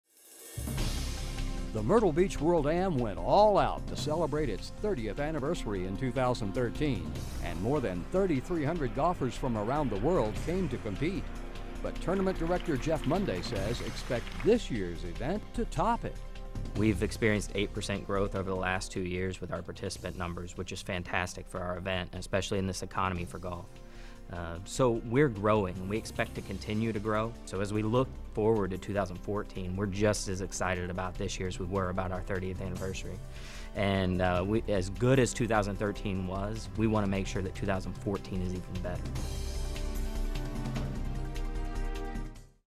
The World's Largest Amateur Golf Tournament
Now in its 31st year, the World Amateur Golf Tournament is a golf event like no other in one of the most popular and enjoyable vacation destinations in America.